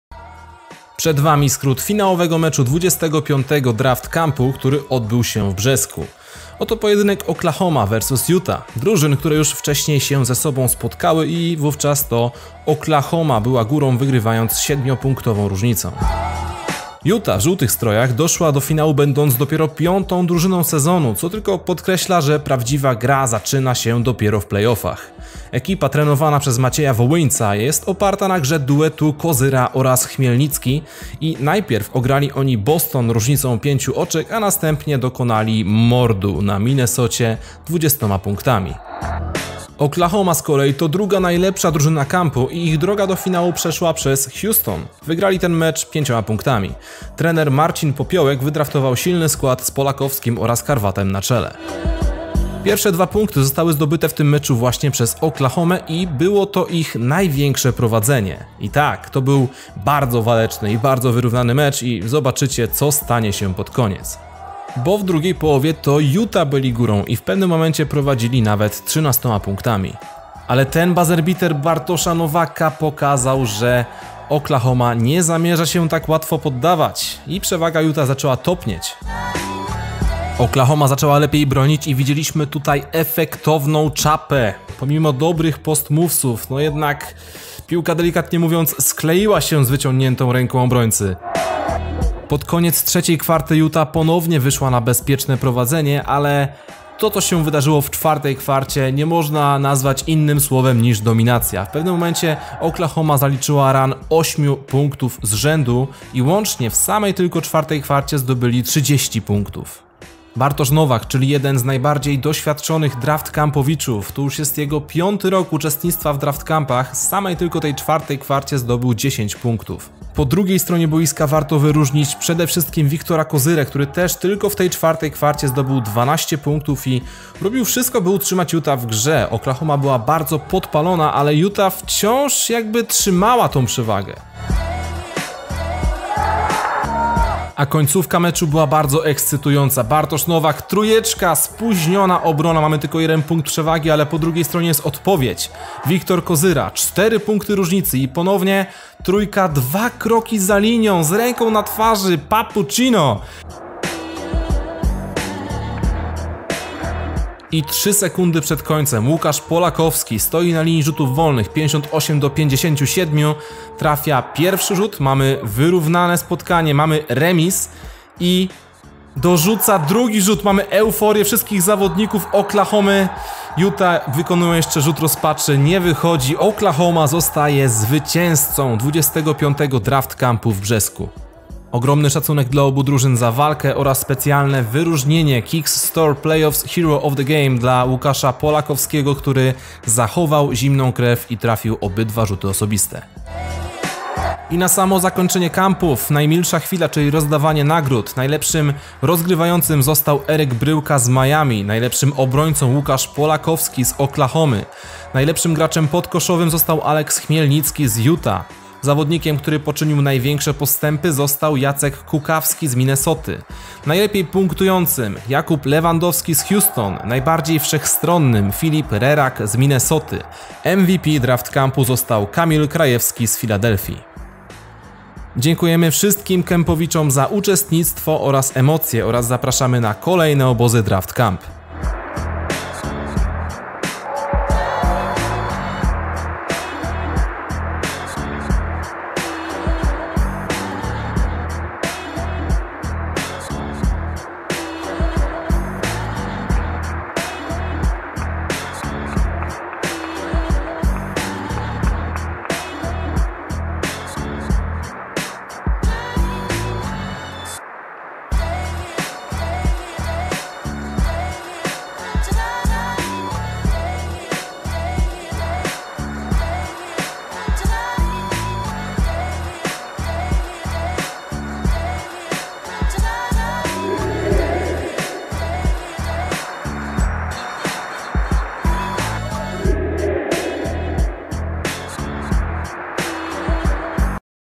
04 07 FINAŁ Oklahoma vs Utah feat. Keepthebeat - Draft Camp 25
OKLAHOMA 59:58 UTAH
Ł.Polakowski 20pkt 5zb 3ast
A.Chmielnicki 21pkt 9zb